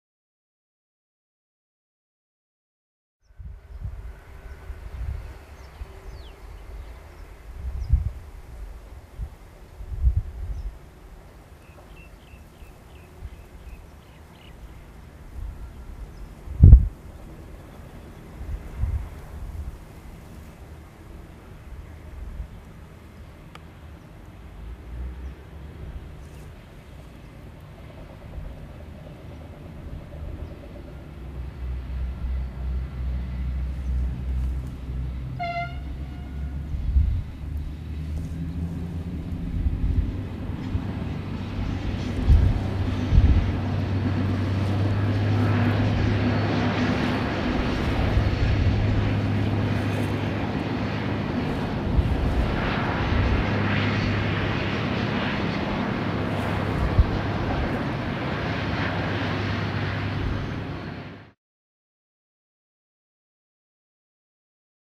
Railways in Australia; V/Line 8111 climbing Ingliston
V/Line Vlocity DMU on train #8111 to Ballarat beginning the 1:48 climb towards Bank Box Loop